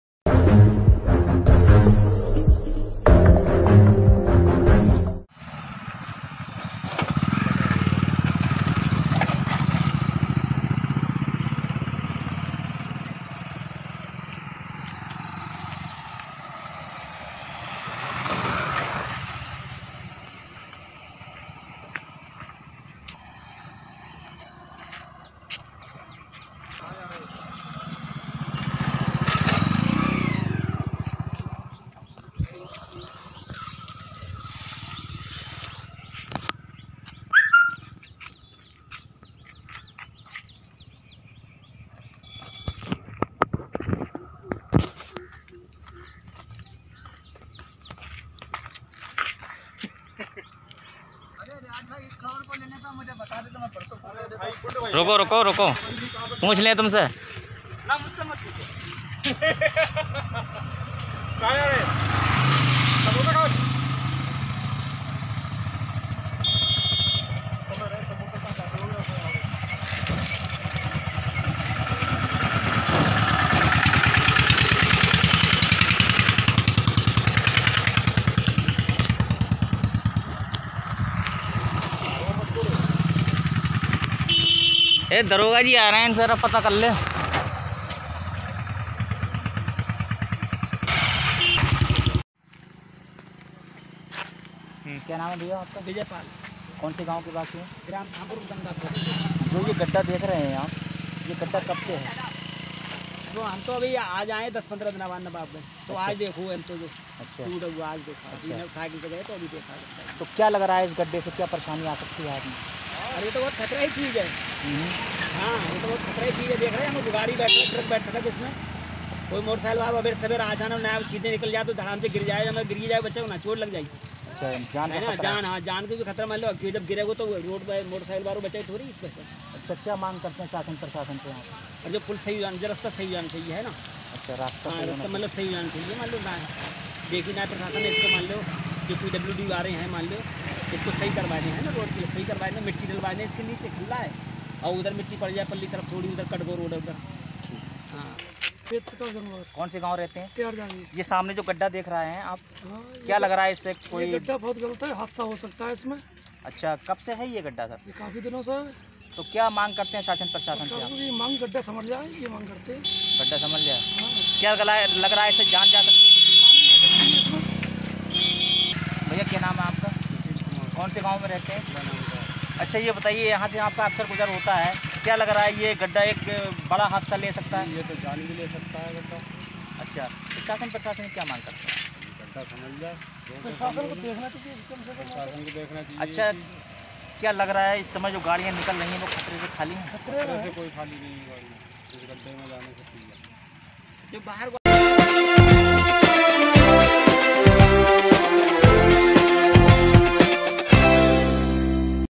Bareilly News : बरेली जान जोखिम में डालकर पुल के ऊपर से निकल रहे हैं राहगीर
बना राहगीरों के लिए जानलेवा गड्ढा ,पुल की वजह से कभी भी हो सकता है एक बड़ा हादसा, शासन और प्रशासन के आला अधिकारी मौन।